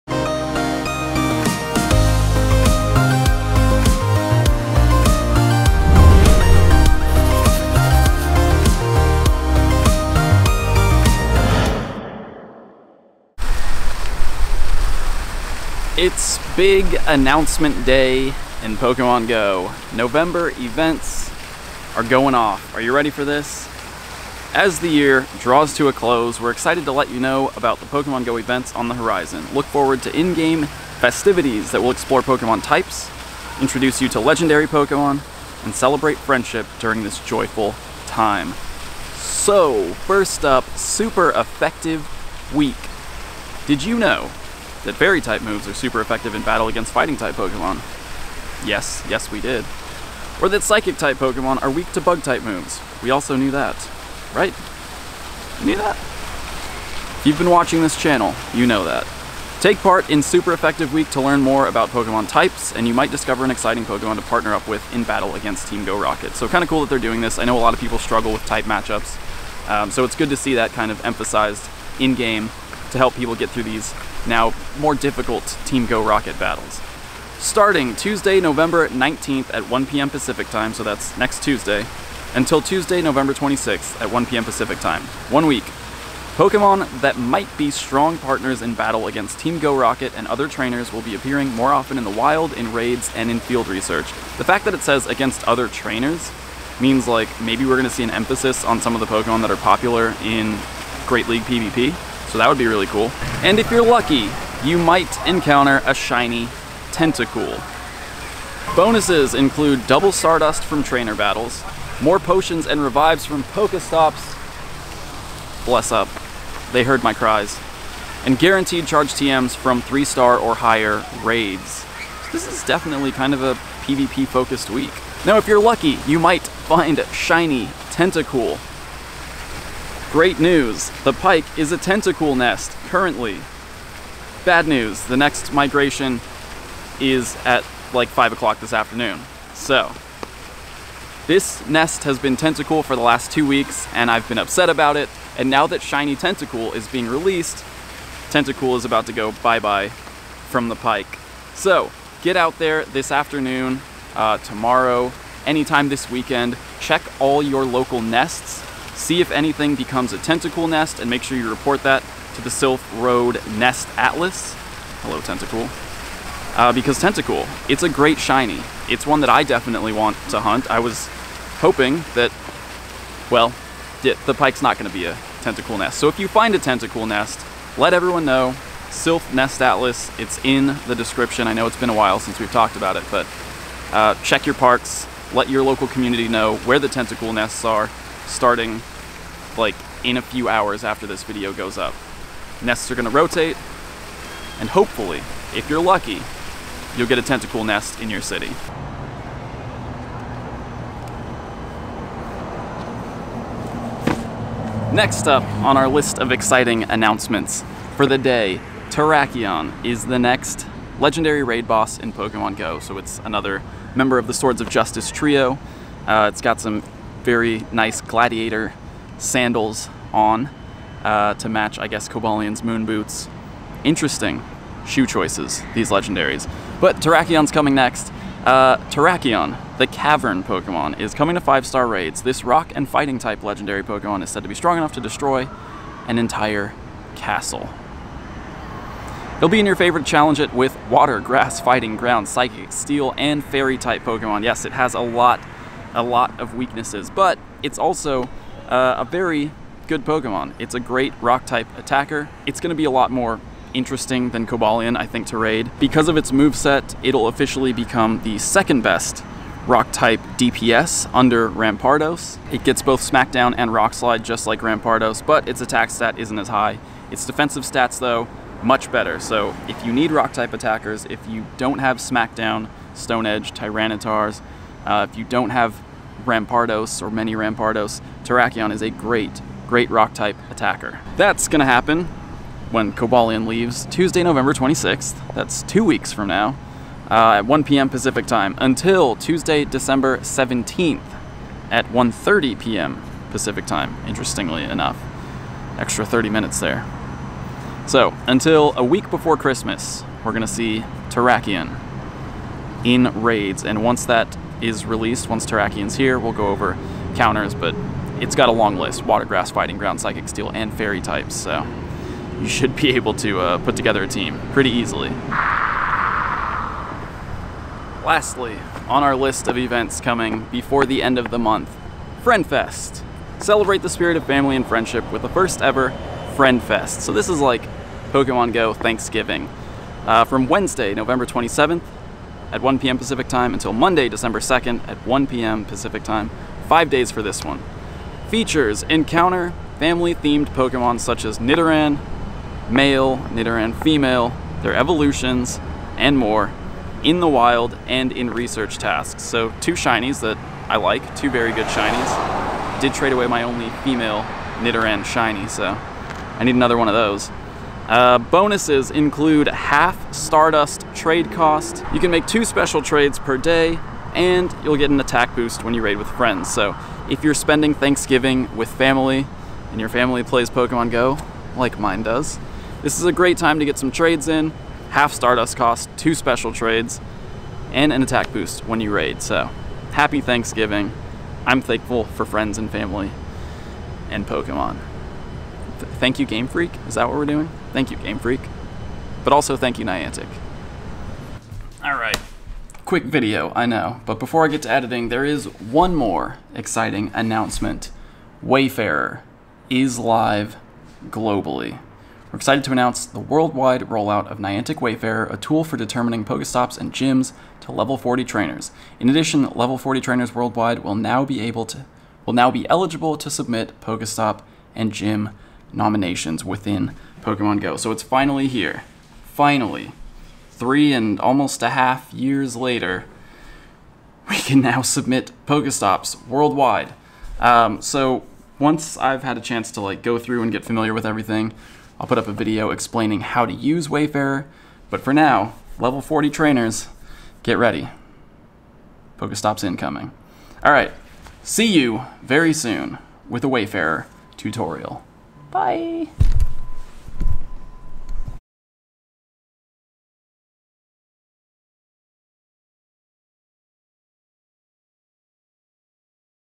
NEXT THREE POKÉMON GO EVENTS! New Shiny Tentacool, New Raid Boss Terrakion + More!
SEND MAIL TO:
8504 Firestone Blvd. # 239

GEAR:
Main Camera - Sony A7iii
Main Lens: 16-35mm f/4